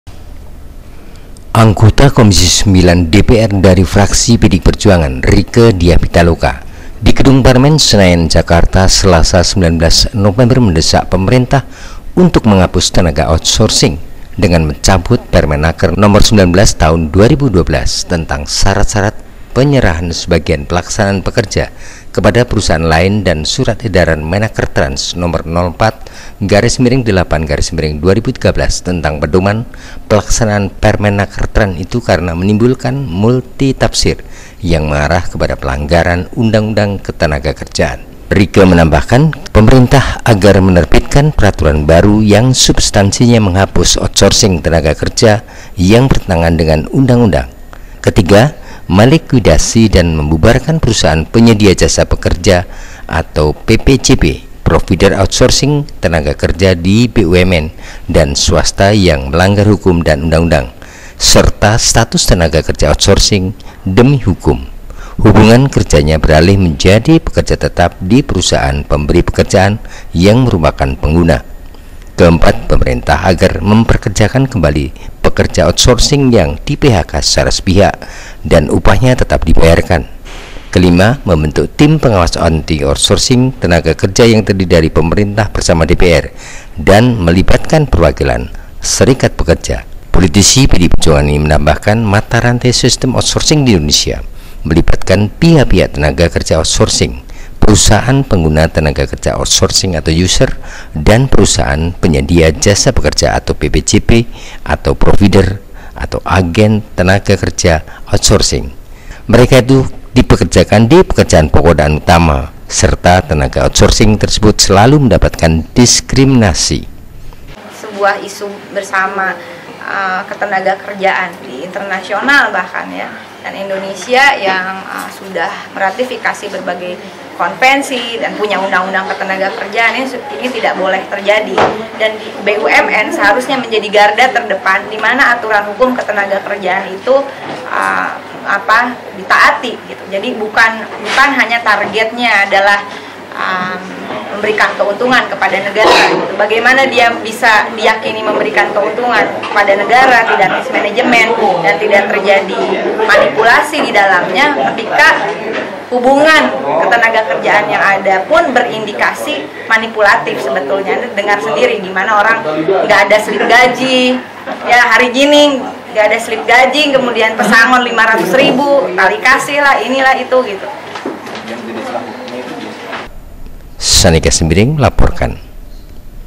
Rieke Diah Pitaloka Hapus Tenaga Kerja Outsourching BUMN
Anggota Komisi IX DPR dari Fraksi PDI Perjuangan Rieke Diah Pitaloka, di gedung Parlemen, Senayan, Jakarta, Selasa (19/11) mendesak pemerintah untuk menghapus tenaga outsourching, dengan mencabut Permenakertrans Nomor 19/2012, tentang syarat-syarat penyerahan sebagian pelaksanaan pekerja kepada perusahaan lain dan surat edaran Menakertrans Nomor 04/VIII/2013 tentang pedoman pelaksanaan Permenakertrans itu karena menimbulkan multitafsir yang mengarah kepada pelanggaran UU Ketenagakerjaan.